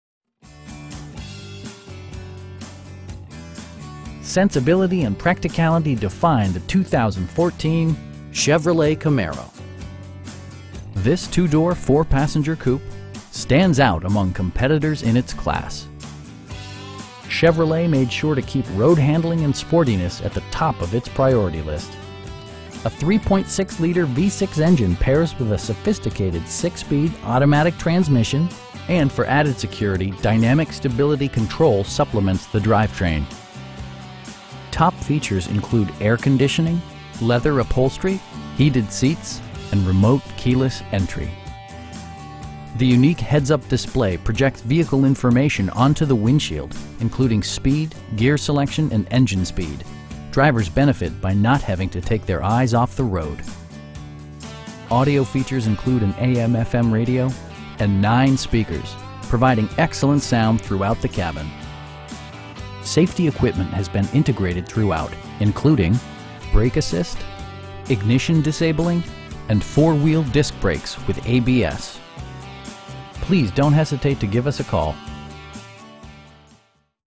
2014 Chevrolet Camaro 2LT in McHenry, IL 60050
Gary Lang Chevrolet
1107 S. Route 31
in McHenry, IL 60050

Sensibility and practicality define the 2014 Chevrolet Camaro. This 2 door, 4 passenger coupe stands out among competitors in its class! Chevrolet made sure to keep road-handling and sportiness at the top of it's priority list. A 3.6 liter V-6 engine pairs with a sophisticated 6 speed automatic transmission, and for added security, dynamic Stability Control supplements the drivetrain. Top features include air conditioning, leather upholstery, 1-touch window functionality, variably intermittent wipers, automatic dimming door mirrors, heated seats, power moon roof, and remote keyless entry. The unique heads-up display projects vehicle information onto the windshield, including speed, gear selection and engine speed. Drivers benefit by not having to take their eyes off the road. With high intensity discharge headlights illuminating your path, you'll always appreciate maximum visibility. Audio features include an AM/FM radio, steering wheel mounted audio controls, and 9 speakers, providing excellent sound throughout the cabin. Safety equipment has been integrated throughout, including: head curtain airbags, front SIDE impact airbags, traction control, brake assist, ignition disabling, onStar, and 4 wheel disc brakes with ABS. We pride ourselves in the quality that we offer on all of our vehicles. Please don't hesitate to give us a call.